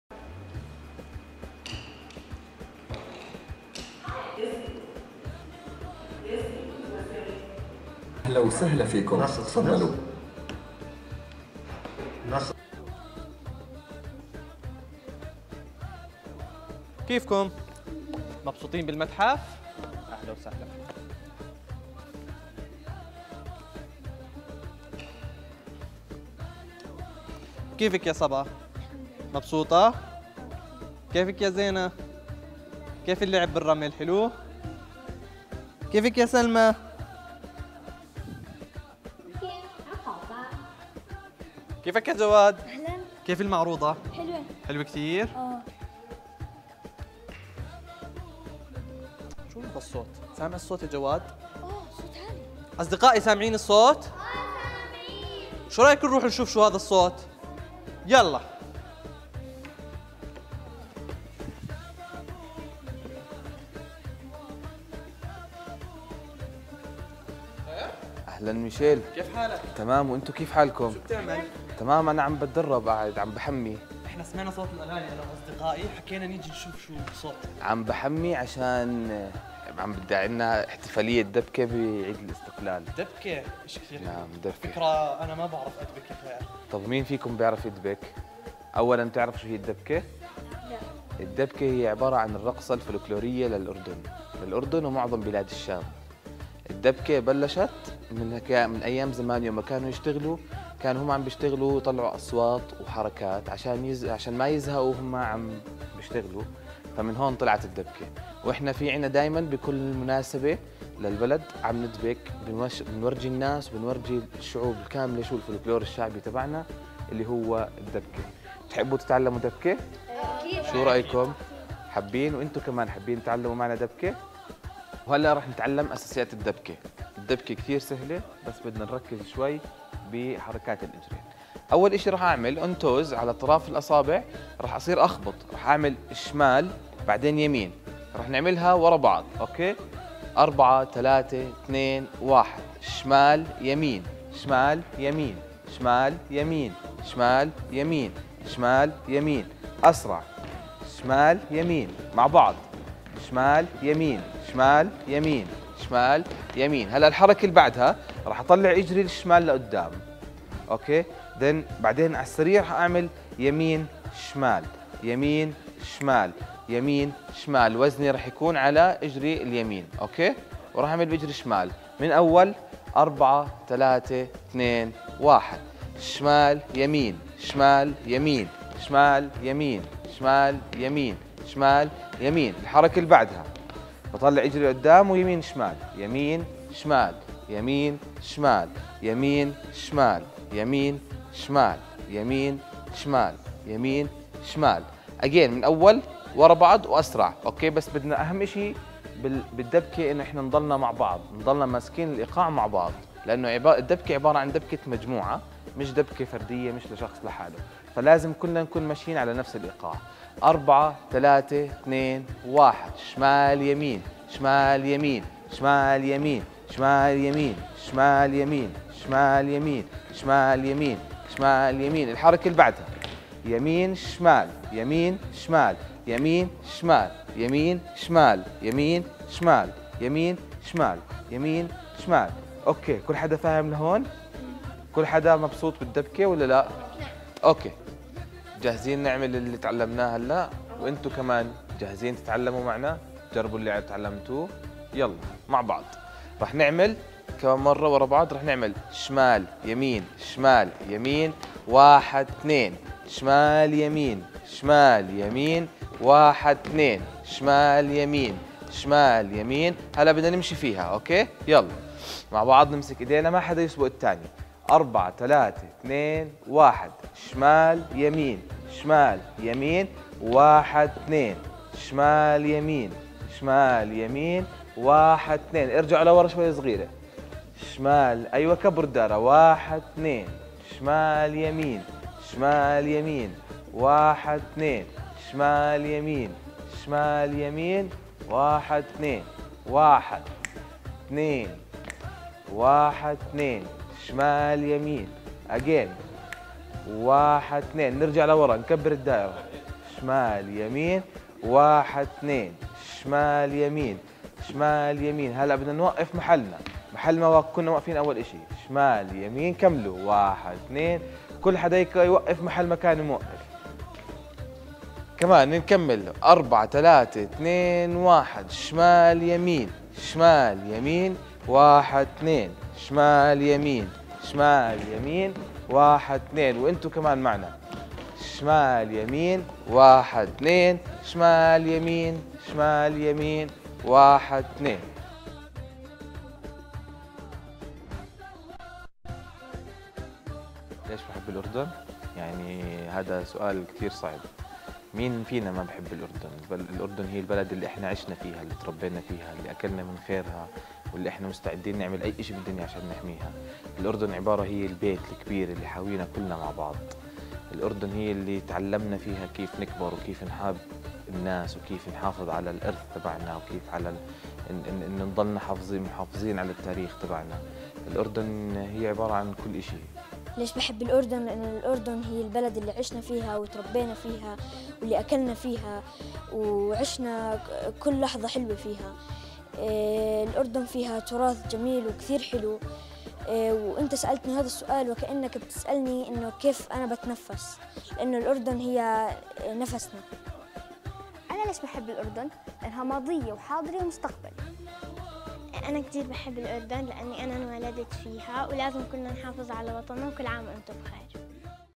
تعلّم الدبكة الشعبية
في هذا الفيديو اجتمع بعض اصدقائنا الصغار مع المدرب ثائر عسكر ليعبروا عن حبهم للأردن، ويتعلموا  تقنيات وأساسيات الدبكة الشعبية التي تشكّل التراث الفلكلوري للأردن.